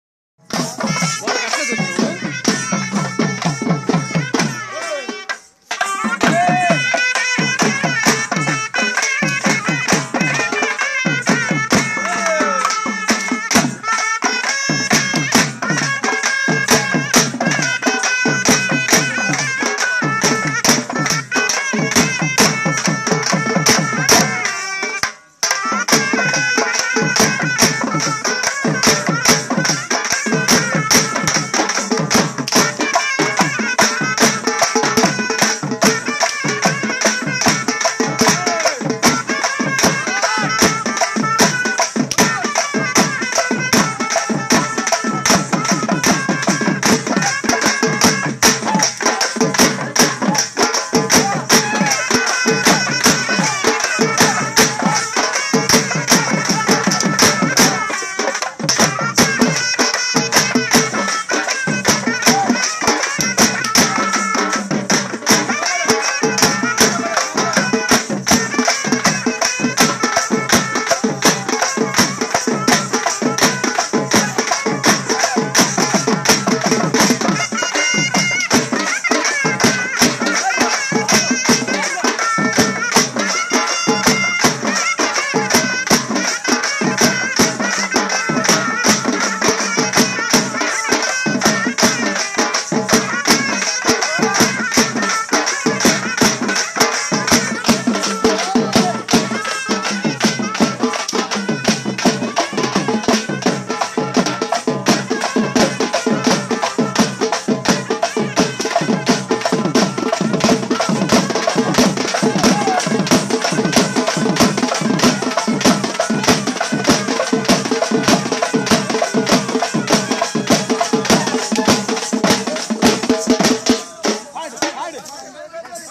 KOLUPULU IN KOPPERAPALEM
In this video my relatives were danced wonderfully to the custom songs in Kolupulu